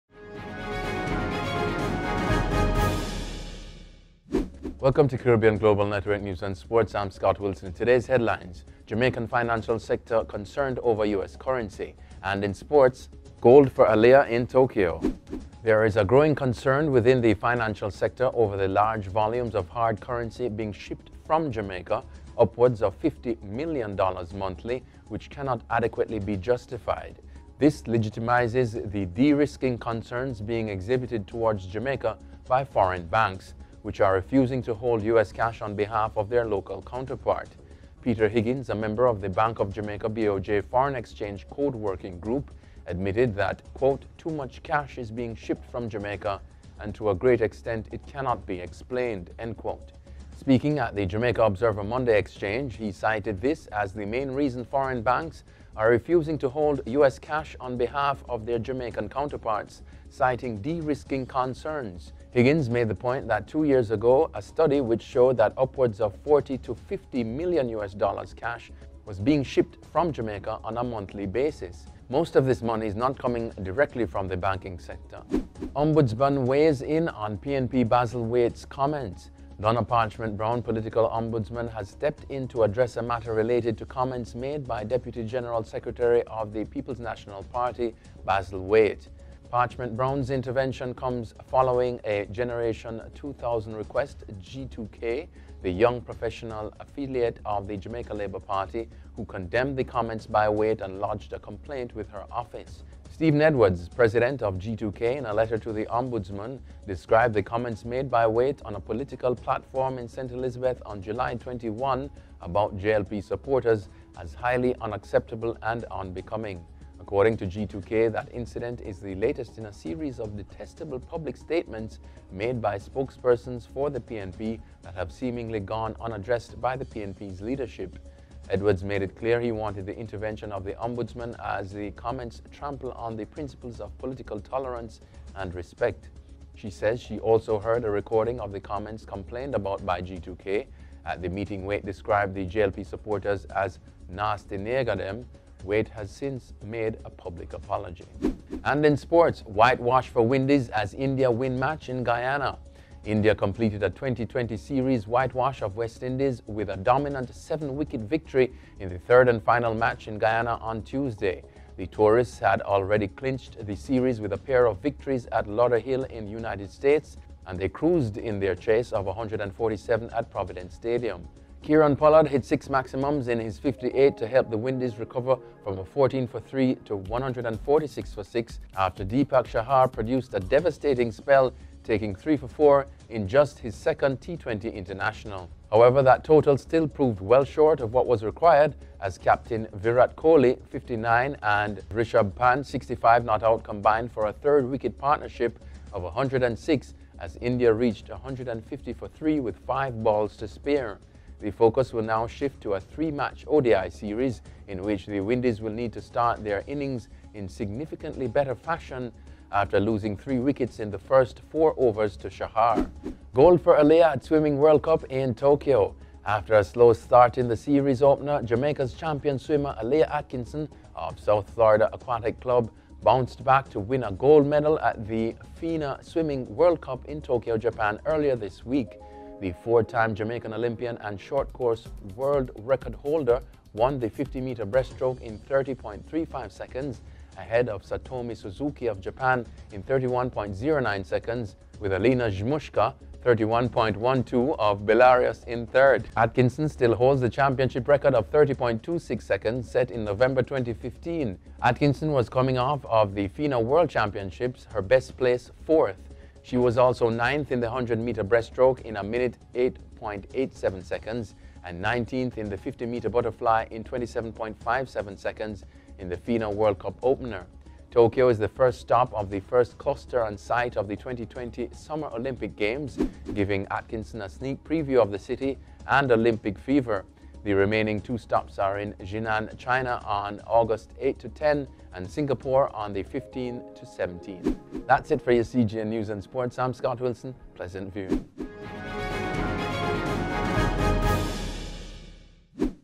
Ombudsman weighs in on PNP Basil Waite's comments - CGN News & Sports Ep 111
Concern as too much US currency being shipped from Jamaica. Six year old whose penis was severed, recovering. Ombudsman weighs in on PNP Basil Waite's comments. ASAJ President Calls For Support For Jamaica’s Divers. Whitewash for Windies as India win match in Guyana. Gold for Alia at Swimming World Cup in Tokyo.